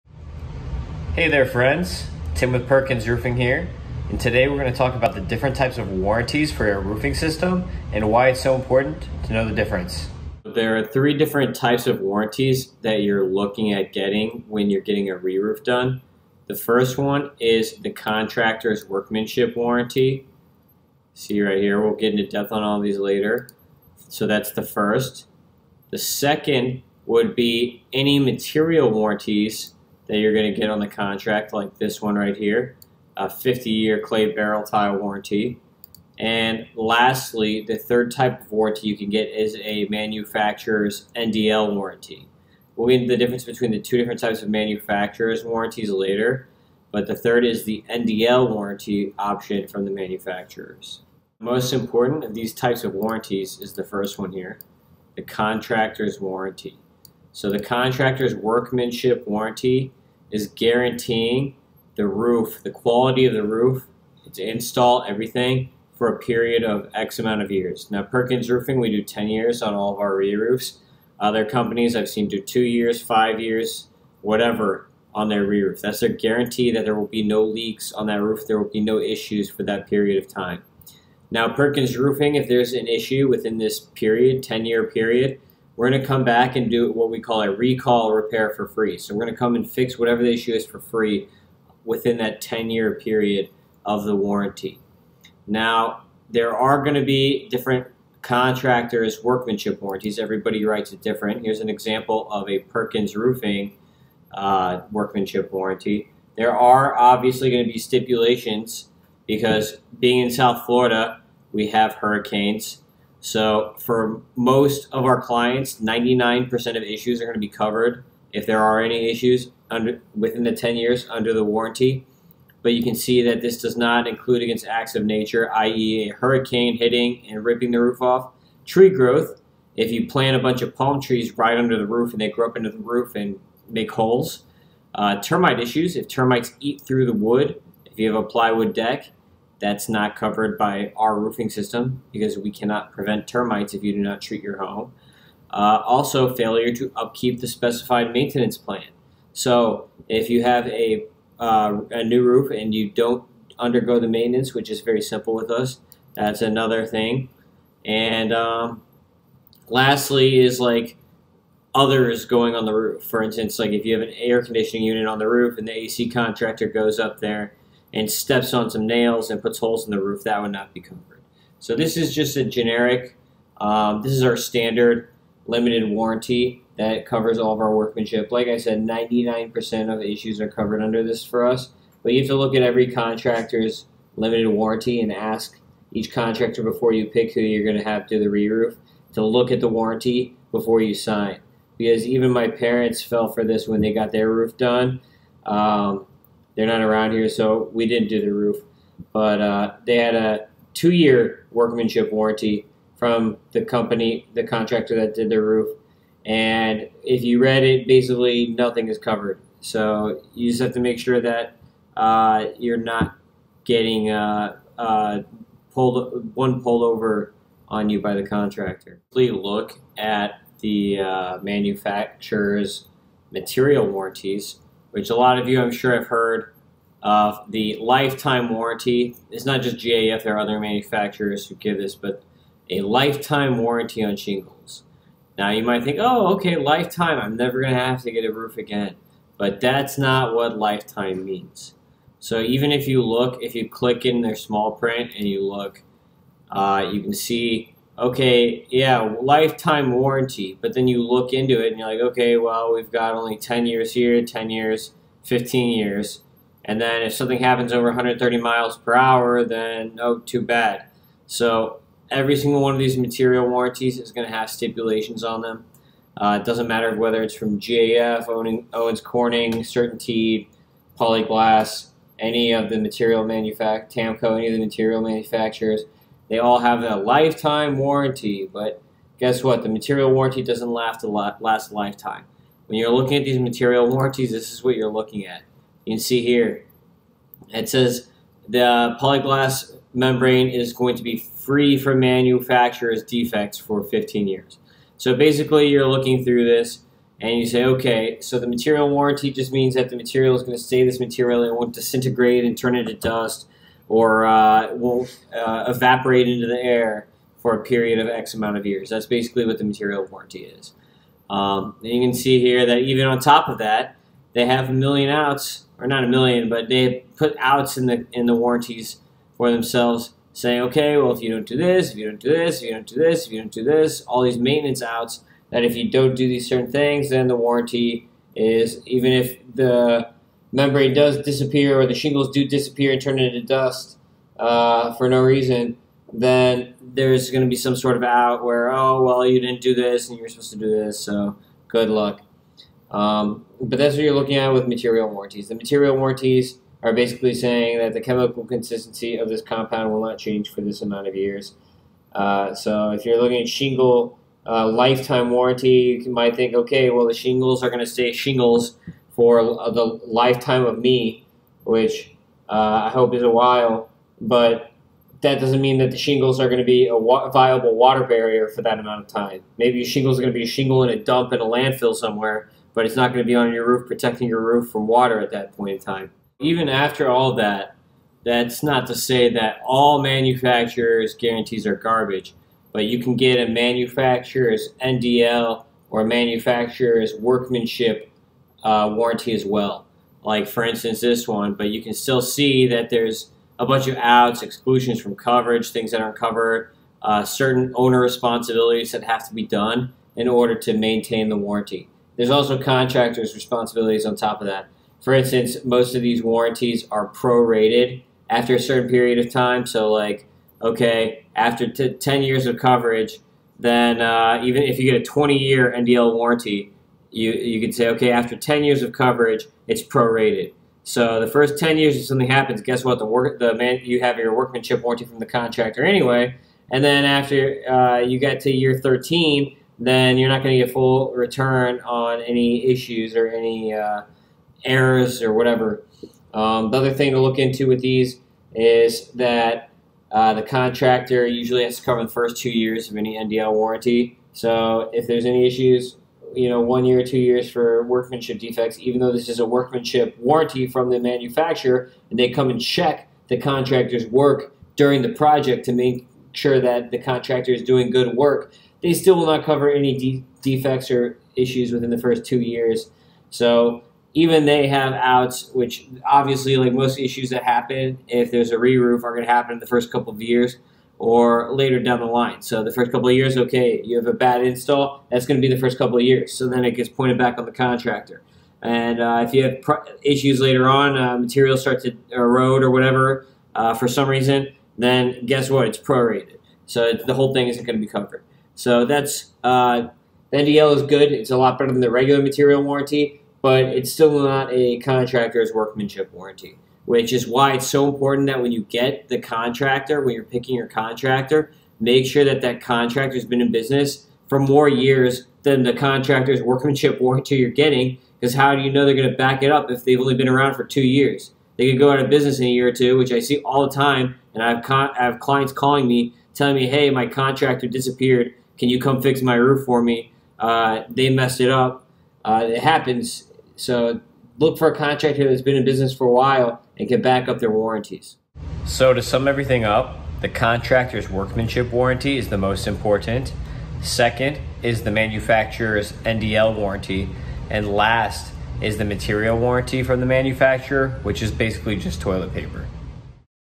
What Are the Different Roofing Warranties? | Perkins Roofing Corp.
Team Perkins Roofing's Tim Kanak teaches us about the three different roofing warranties you can expect in a reroof project. It's important to know the differences between each, and what it means for you and your new roof.

1.) Contractors' workmanship warranty
2.) Any material warranties
3.) Manufacturer's No Dollar Limit (NDL) warranty

Complimentary estimates!  📞 305-MIA-ROOF
Serving the entire southeast Florida area from Palm Beach County down to Monroe County. 🌴